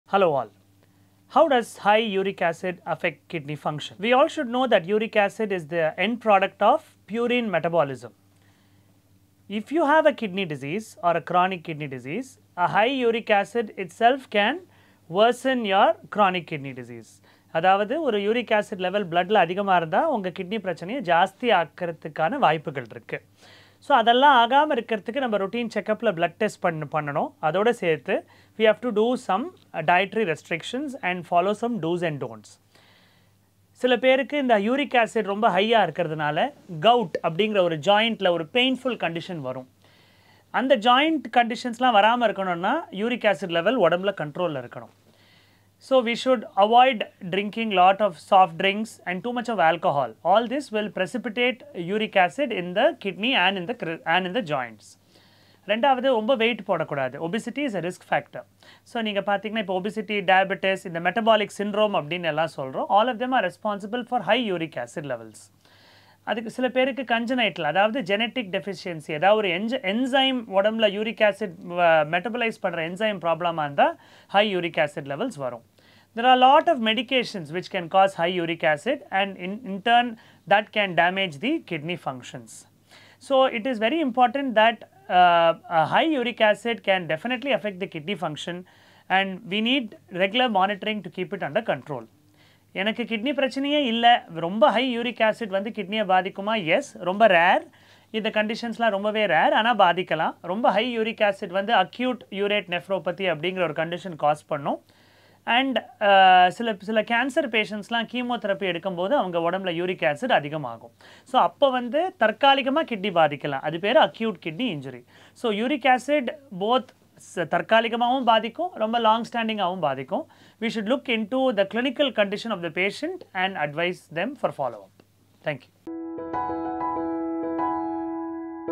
Can high uric acid affect kidney function? | அதிக யூரிக் ஆசிட் சிறுநீரகத்தை பாதிக்குமா?
Uric acid is the end product of purine metabolism. If a patient has chronic kidney disease, high uric acid can worsen the condition. High uric acid can cause gout in some people.

To avoid this, it is important to routinely take blood tests along with dietary restrictions.
Avoid consuming soft drinks since they can precipitate uric acid in the kidney and the joints.

Maintain the ideal weight since obesity is a risk factor.

Watch Dr. Balaji Kirushnan, Nephrologist from Kauvery Hospital talk about the link between uric acid levels and kidney diseases.